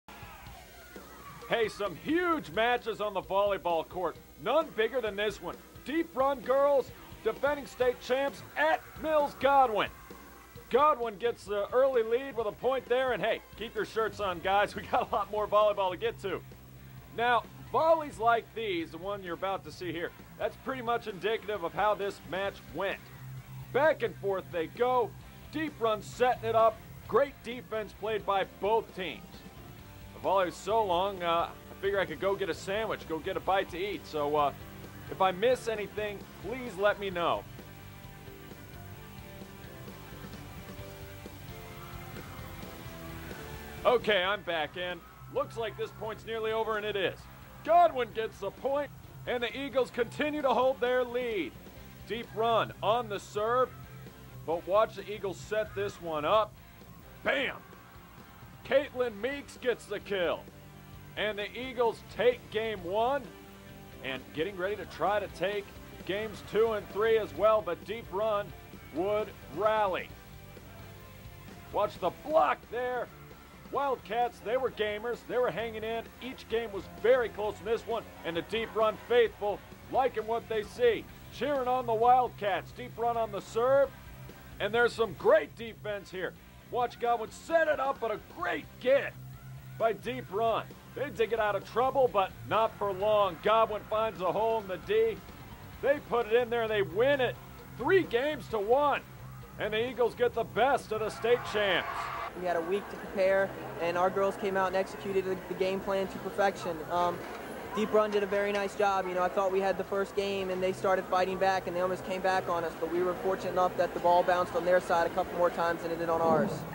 Godwin vs Deep Run Girl's Volleyball
Girl's high school volleyball game between Godwin and Deep Run in Henrico County